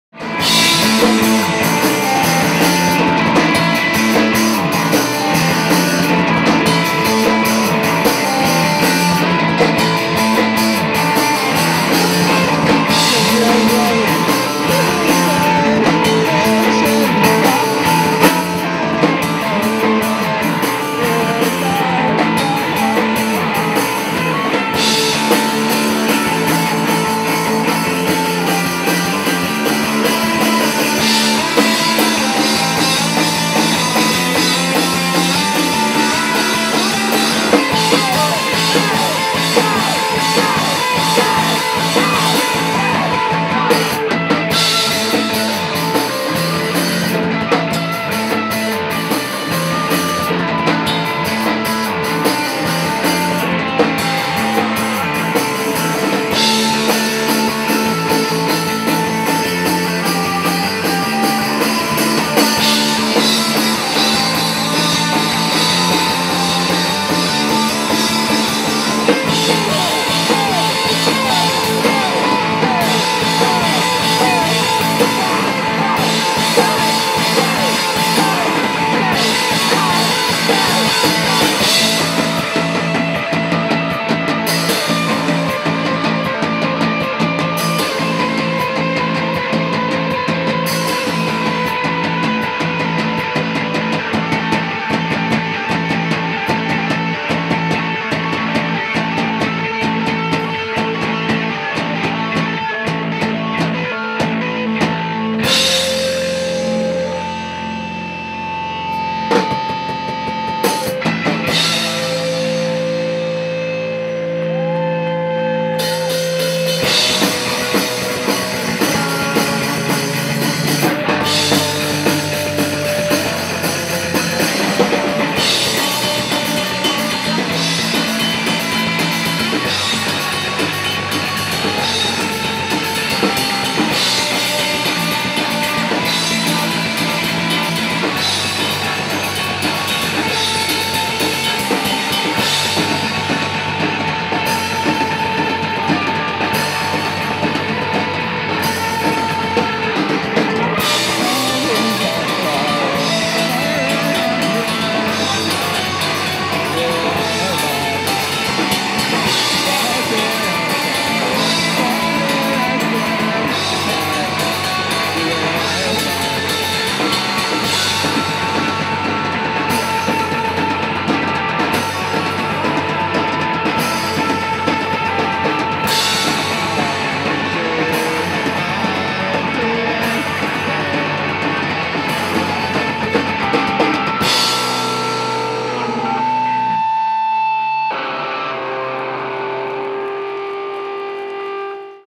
Ultrasphinx - Highland Square Porch Rokr 2013
Ultrasphinx performing at the inaugural edition of the Highland Square Porch Rokr Festival on June 8, 2013. Thank you to Ralph for hosting this performance. Video recording by Terrapin Media in Highland Square.